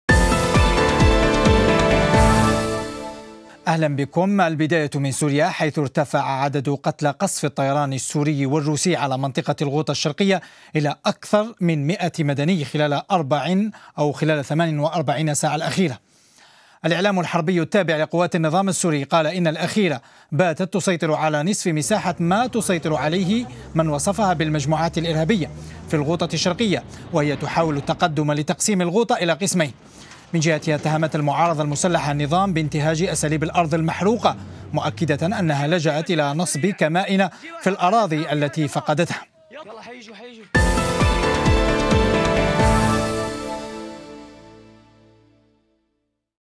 أكثر من 100 قتيل في غوطة دمشق الشرقية خلال 48 ساعة
ارتفعت حصيلة قصف الطيران السوري والروسي على الغوطة الشرقية المحاصرة بريف دمشق إلى أكثر من مئة مدني خلال 48 ساعة.

الإعلام الحربي التابع لقوات النظام السوري قال إن الأخيرة باتت تسيطر على نصف مساحة ما تسيطر عليه من وصفها بـ"المجموعات الإرهابية" في الغوطة الشرقية، وهي تحاول التقدم لتقسيم الغوطة إلى قسمين.